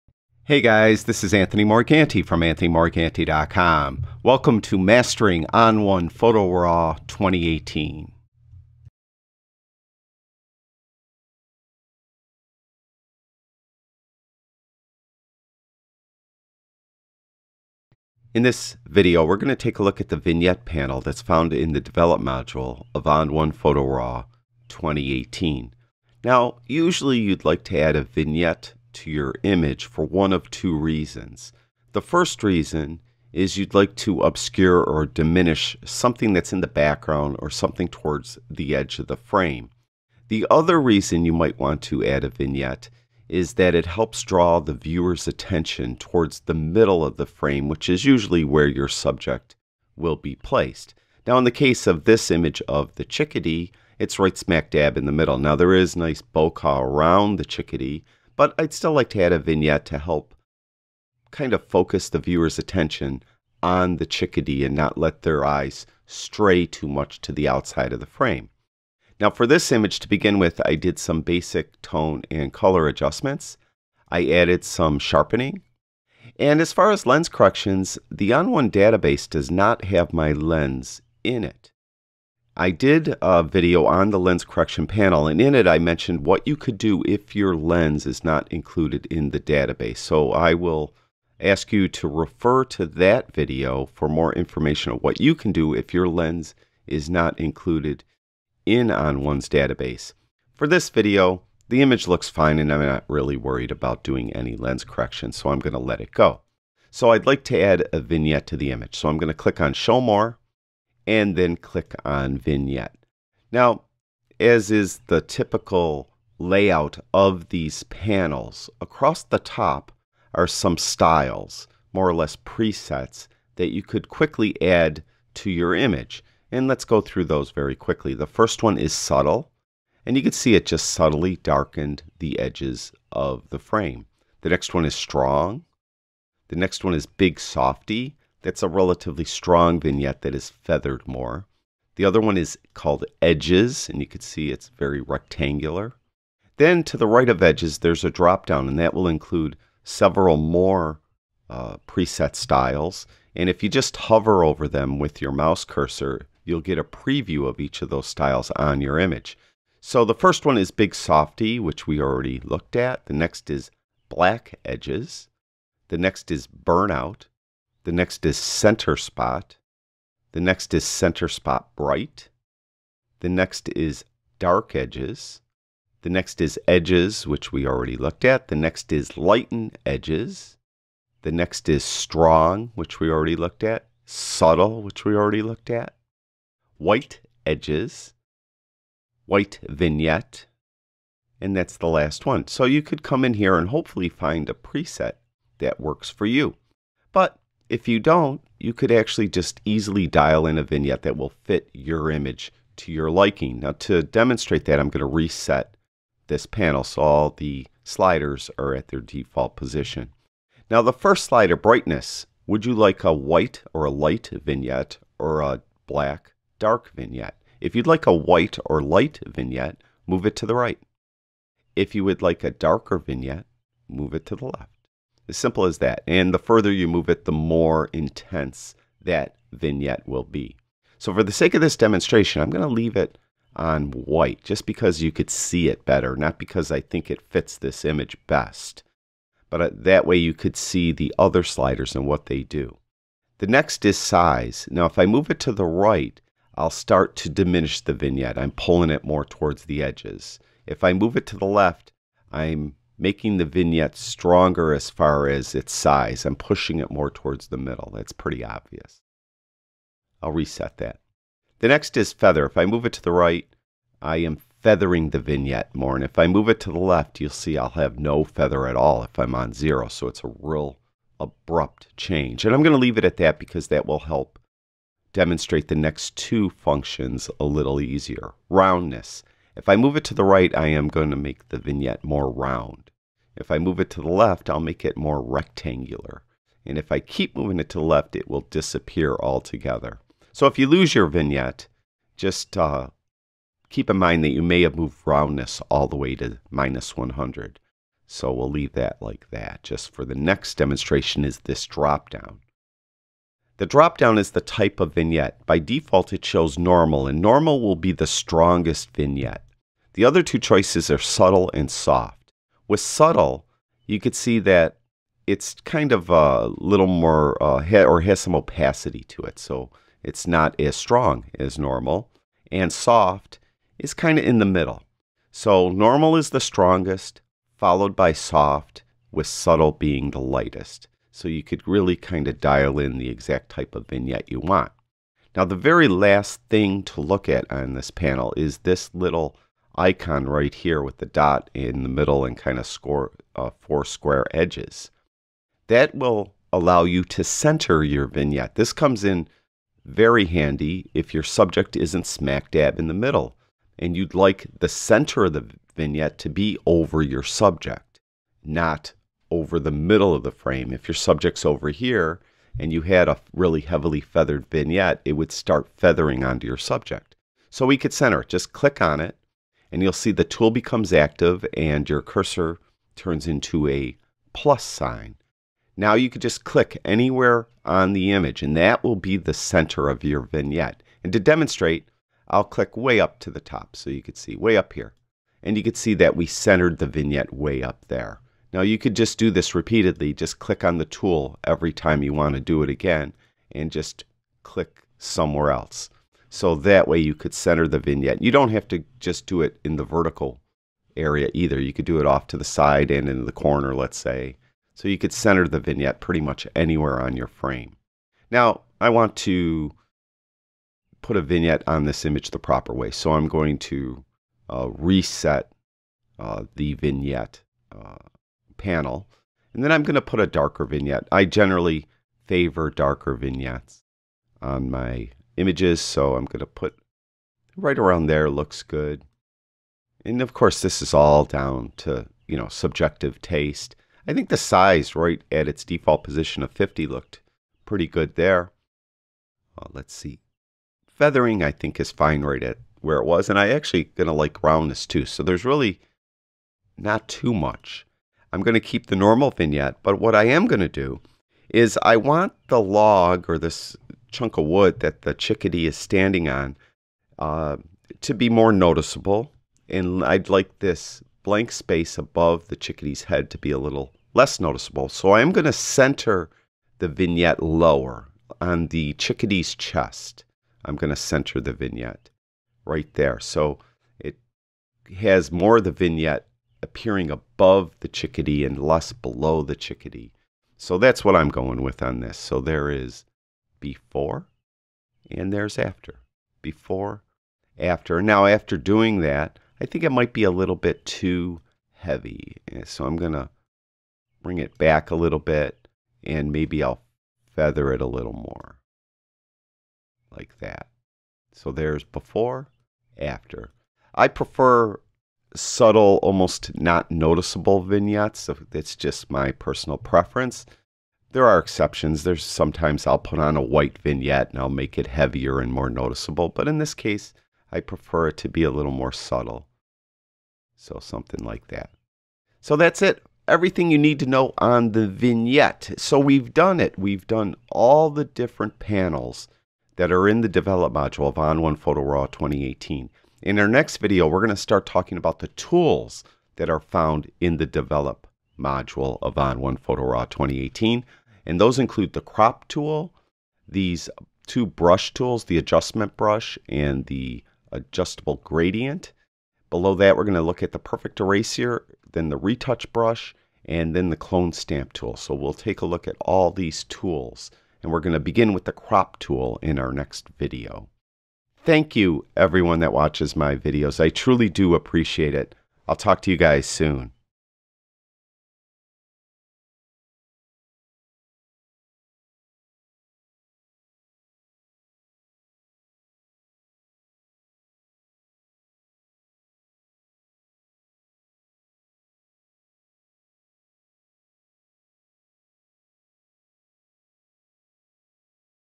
Mastering On1 Photo RAW 2018 - Episode 17: Vignette Panel
In this, Episode 17 of Mastering On1 Photo RAW 2018, I go over in detail, how to use the Vignette Panel -- with it, you'll be able to add a custom vignette to your image in order to diminish something in the background and/or to draw the viewer's attention to your subject.

AM17

Coming Soon:

Morganti On1 Photo RAW Presets
The RAW Files Used in These Videos
On1 Photo RAW Cheat Sheets

I'll also be making all the videos for the series that are on YouTube available for download.